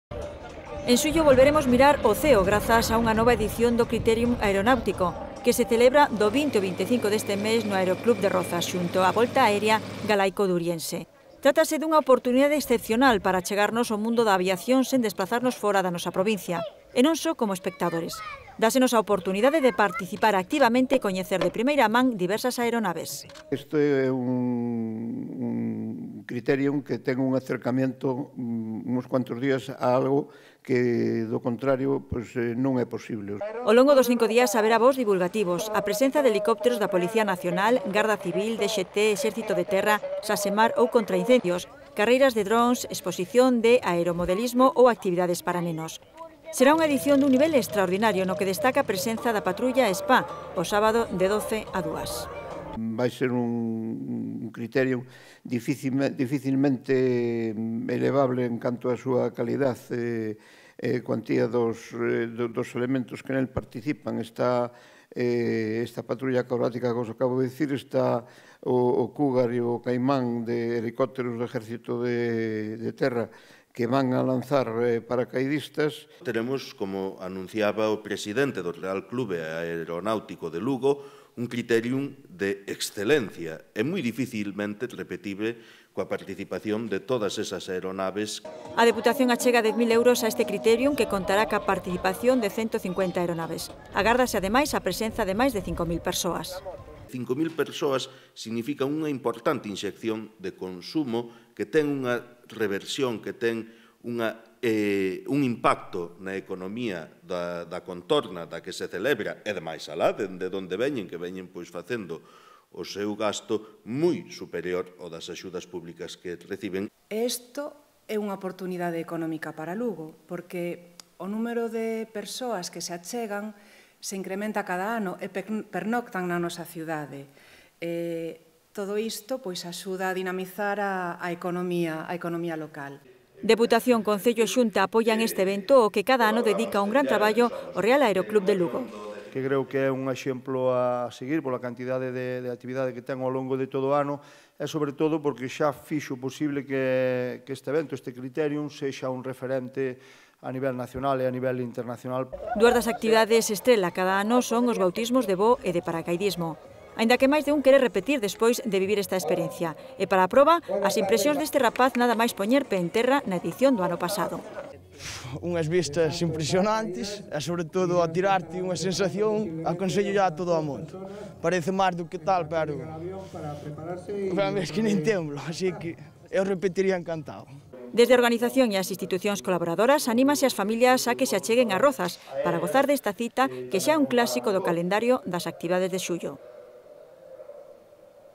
Presentación XIII Critérium Aeronáutico de Lugo
En xullo volveremos mirar ao ceo grazas a unha nova edición do Critérium Aeronáutico que se celebra do 20 ao 5 deste mes no Aeroclub de Rozas xunto a volta aérea Galaico-Duriense. Deputación, Concello de Lugo e Xunta apoian este evento ao que cada ano dedica un gran traballo o Real Aeroclub de Lugo.